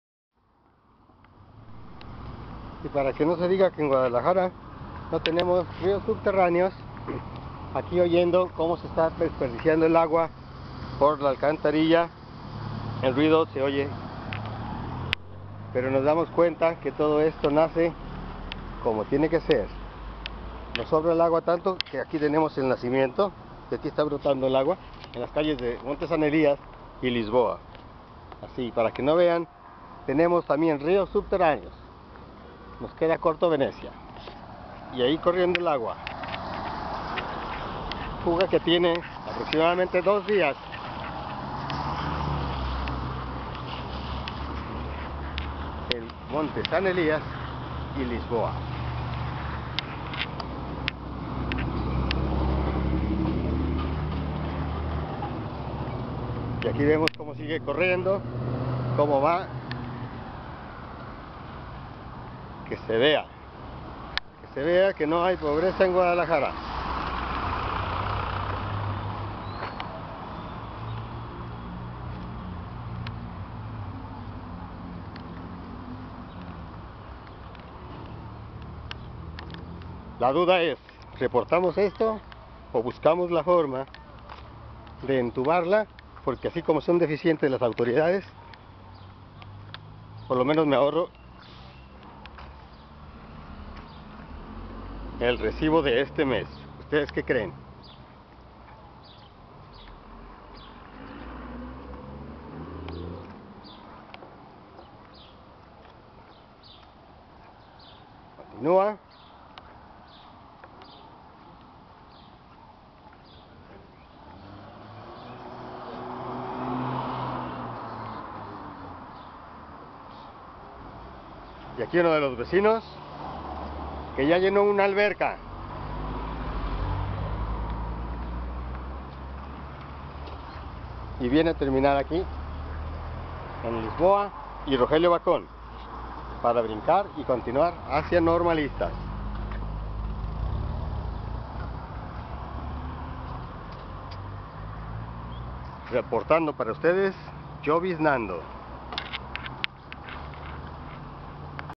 INUNDACION Y DUDA EN GUADALAJARA. SAN ELIAS Y LISBOA
Lo reporto o pongo una tubería para ahorrarme el agua el de este mes o quizás mas ..ja ja ja ja como son tan eficientes los del agua pero solo para cobrar...!